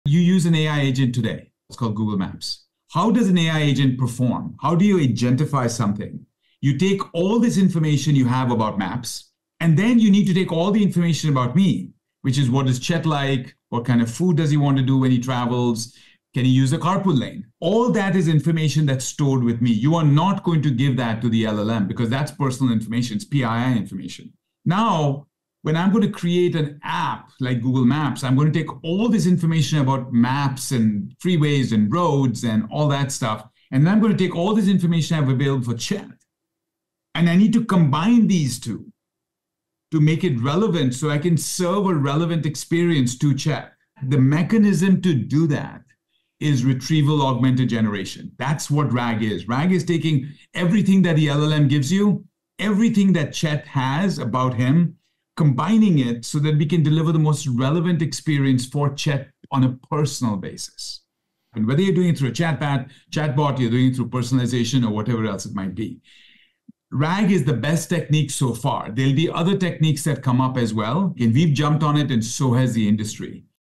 You use an AI agent today. - Chet Kapoor
You use an AI agent today. It's called Google Maps. How does an AI agent perform? How do you agent-ify something?

You take all this information you have about maps. And then you need to take all the information about me. Which is what is Chet like? What kind of food does he want to do when he travels? Can he use the carpool lane?

All that is information that's stored with me. You are not going to give that to the LLM because that's personal information it’s PII information. [personally identifiable information)

Now when I’m going to create an app like Google Maps, I’m going to take all of this information about maps and freeways and roads and all that stuff. And then I'm going to take all this information I have available for Chet.

And I need to combine these two to make it relevant so I can serve a relevant experience to Chet. The mechanism to do that is ‘retrieval augmented generation’ [RAG] That's what RAG is.

RAG is taking everything that the LLM gives you, everything that Chet has about him, combining it so that we can deliver the most relevant experience for Chet on a personal basis. And whether you're doing it through a chatbot, your doing it through personalization or whatever else it might be.

RAG is the best technique so far. There'll be other techniques that come up as well. And we've jumped on it and so has the industry.

Chet Kapoor: GenAI, My Agent, Human-like, Reinvention | Work 20XX podcast with Jeff Frick Ep32
- or wherever you podcast